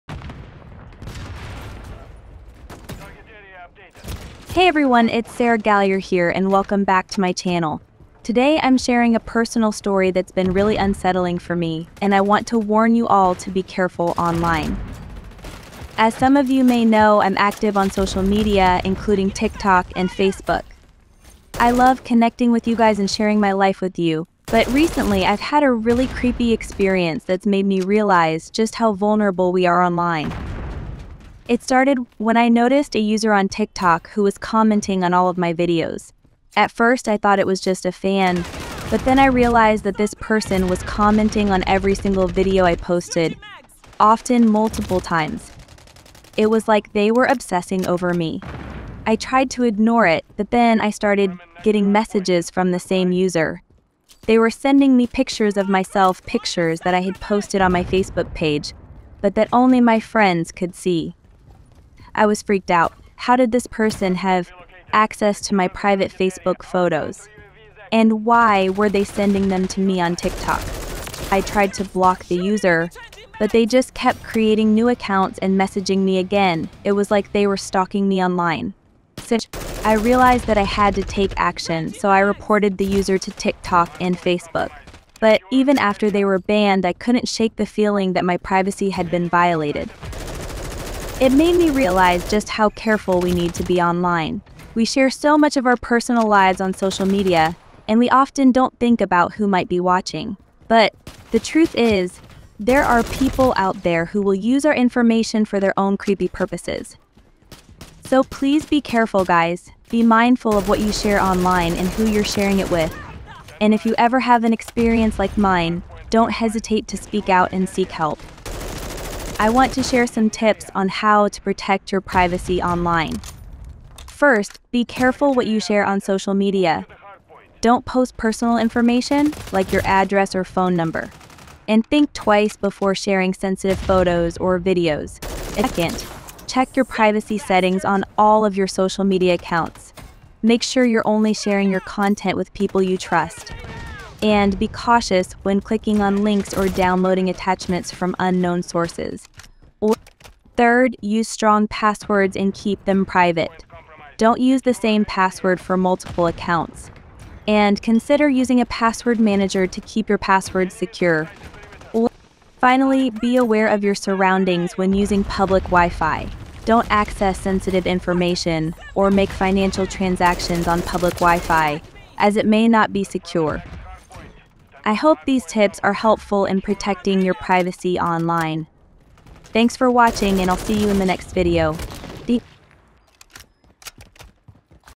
My Creepy Online Stalking Experience - A Warning to Be Careful Online
Today, I'm sharing a personal story that's been really unsettling for me, and I want to warn you all to be careful online.

I recently had a creepy experience with an online stalker who was able to access my private Facebook photos and send them to me on TikTok. It was a violation of my privacy and it made me realize just how vulnerable we are online.

In this video, I'm sharing my story and warning you all to be mindful of what you share online and who you're sharing it with. It's important to be careful and take steps to protect your privacy.